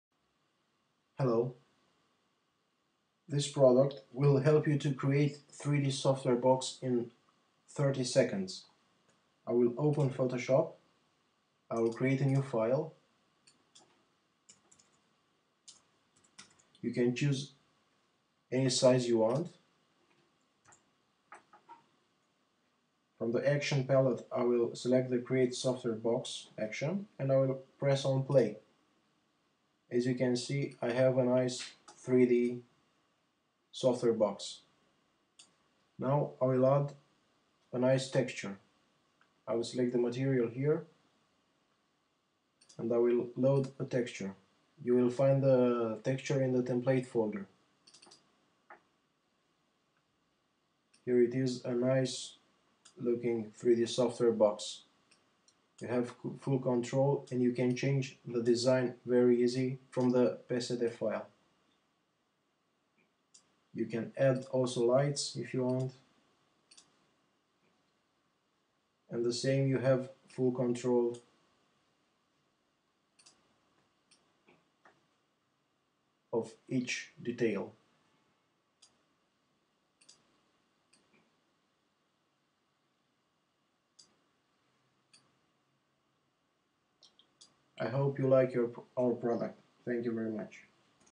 How to Create 3D Software box in under 30 seconds in Adobe photoshop CS5
This video will show you a powerful products available on graphstock.com which allows you to create 3D software boxes in Photoshop without knowing 3D at all. You can rotate, flip, roll, move, scale your software box with a few clicks. You can promote your products with style, and you can increase your conversions if you have nice looking 3D boxes.